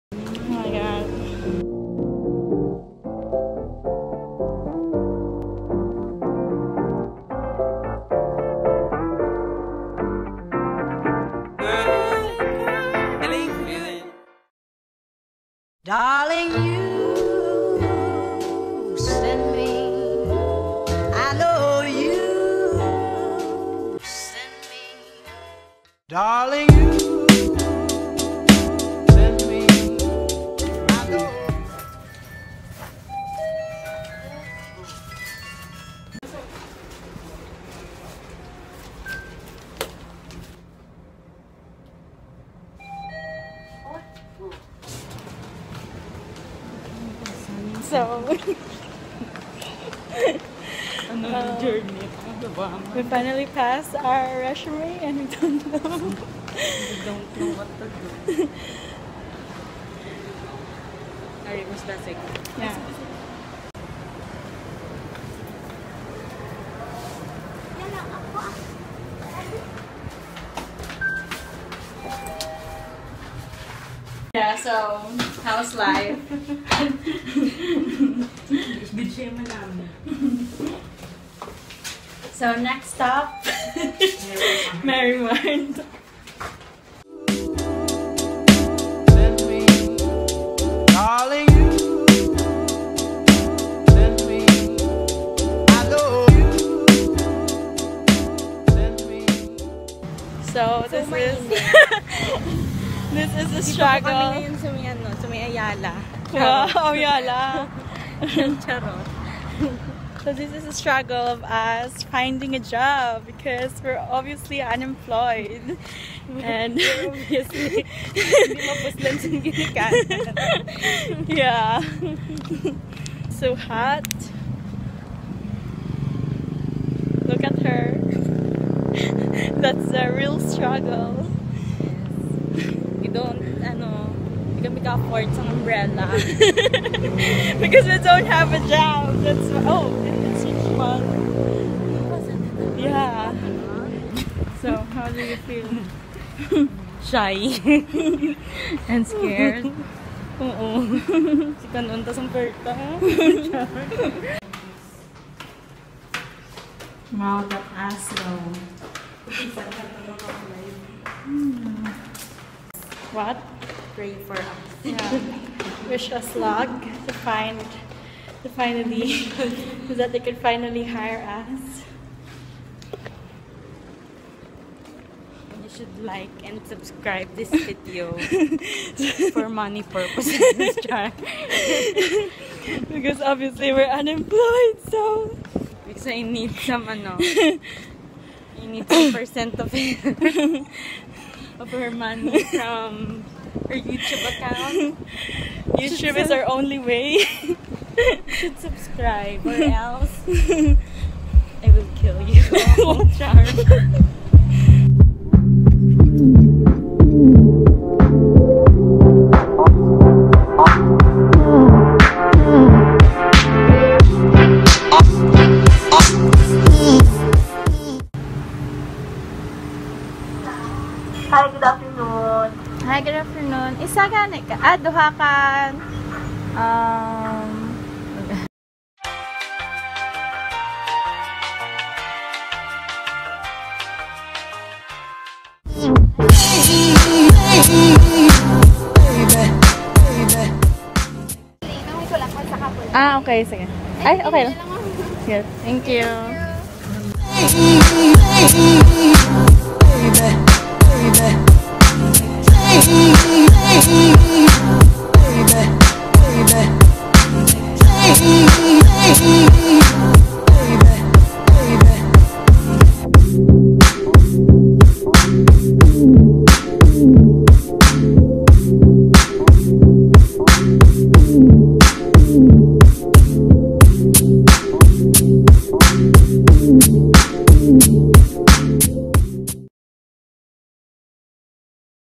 A Day in the Life of an UNEMPLOYED PERSON ( looking for a job vlog )
TWITTER & INSTAGRAM - @lorenalegroso

ATTENTION COMPANIES:
Companies only please!

ABOUT ME:
 Loren Ann Alegroso
January 4, 1999
20 years old